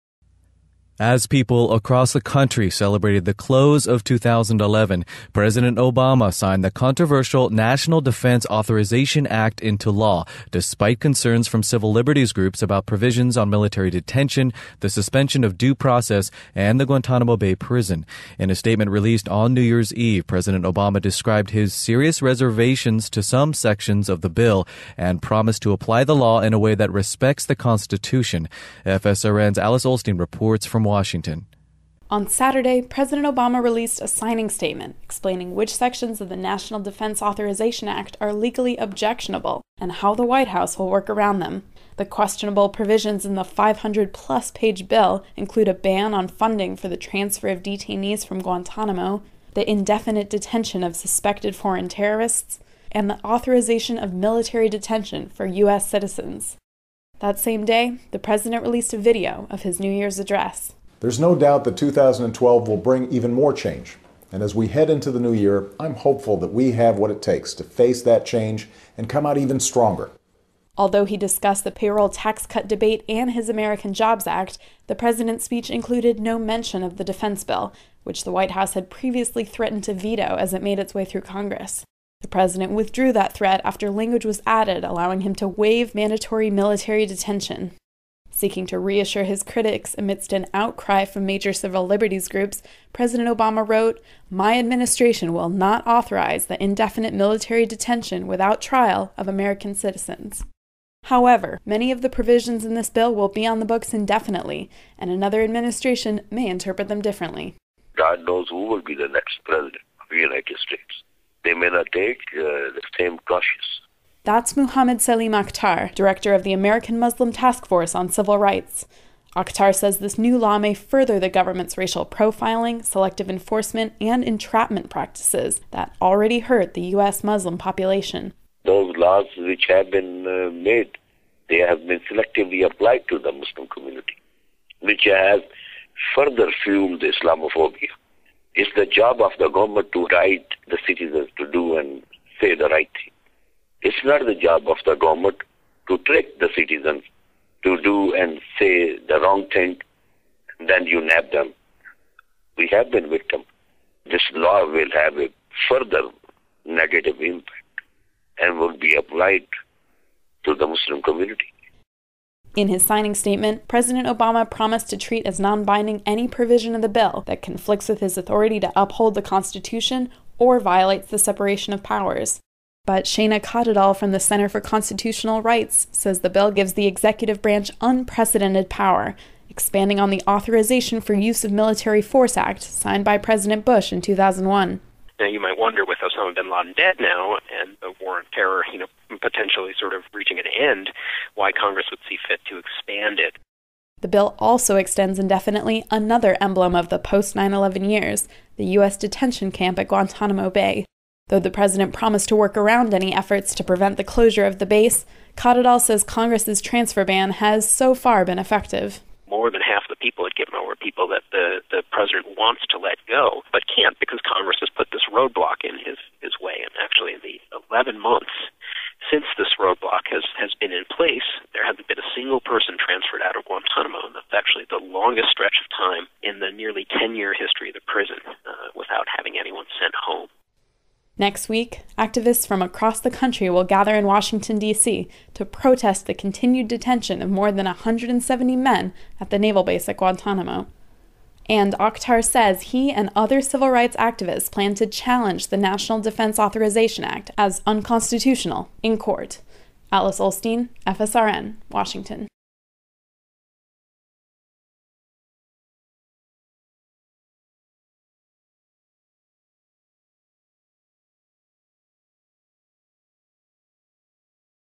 FSRN Obama Signs National Defense Authorization Act Into Law
As people across the country celebrated the close of 2011, President Obama signed the controversial National Defense Authorization Act into law, despite concerns from civil liberties groups about provisions on military detention, the suspension of due process and the Guantánamo Bay prison. In a statement released on New Year's Eve, President Obama described his serious reservations to some sections of the bill, and promised to apply the law in a way that respects the Constitution. FSRN's Alice Ollstein reports from Washington.